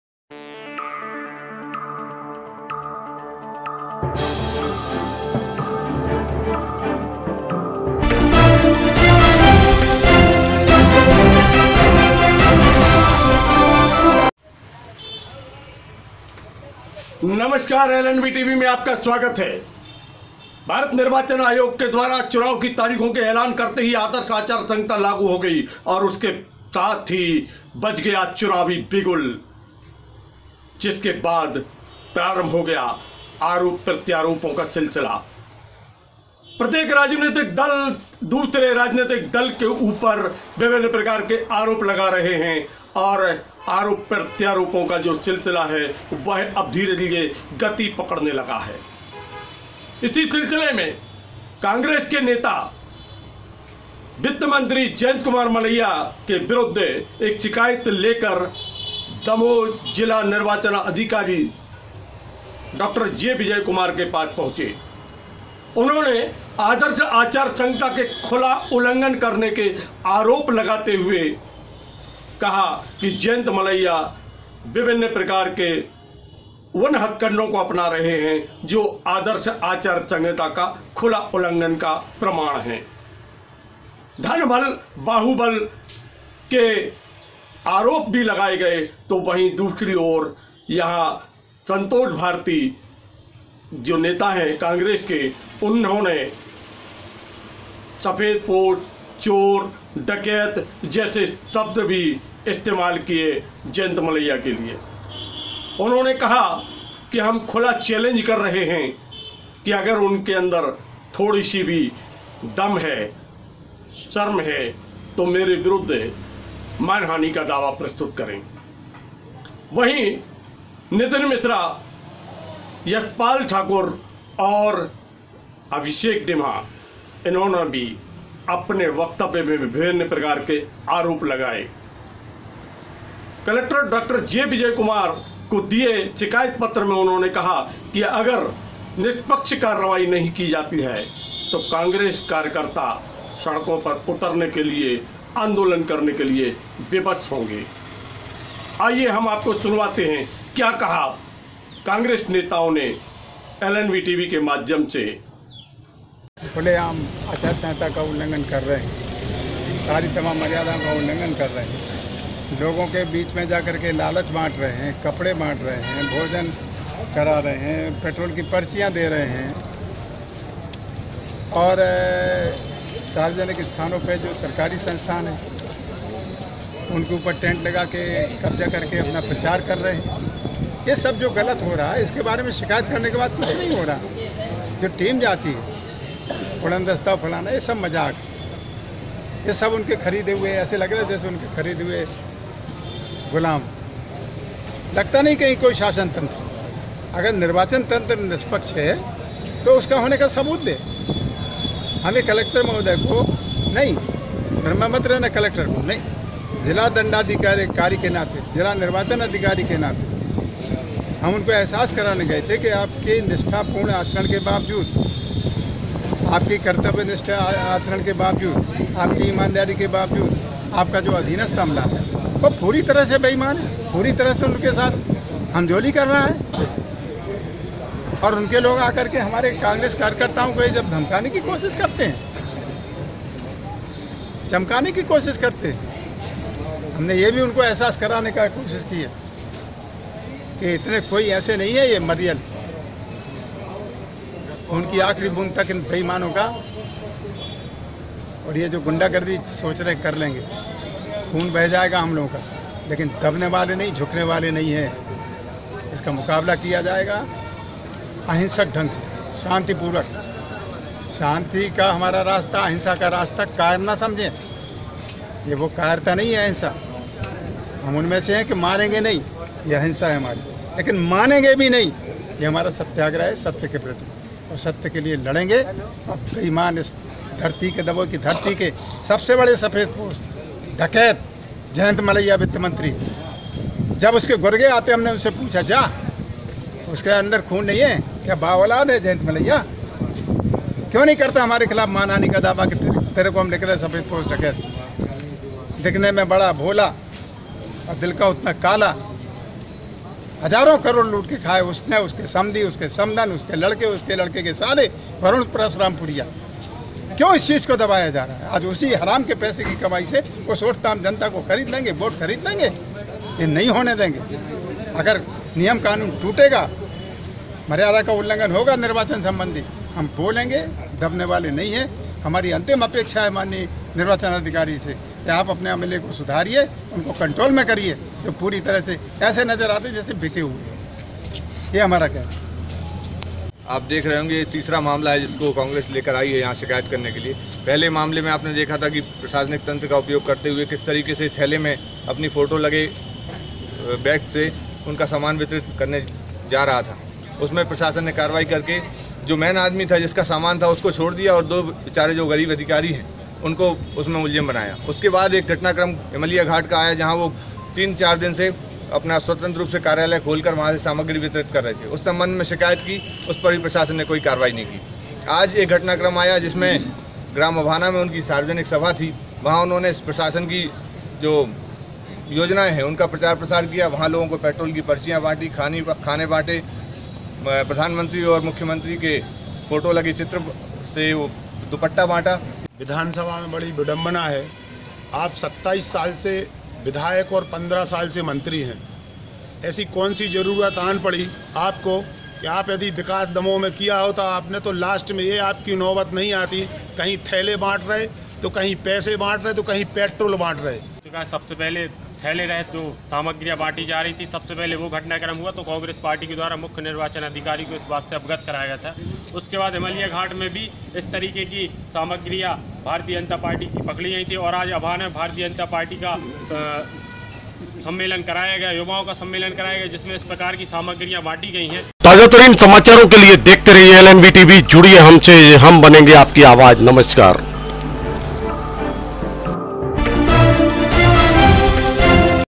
मंत्री मलैया पर कांग्रेस नेताओं ने लगाया अचार संहिता उलंघन का आरोप
LNV Tv 12 Oct 2018 ( मंत्री मलैया पर कांग्रेस नेताओं ने लगाया अचार संहिता उलंघन का आरोप )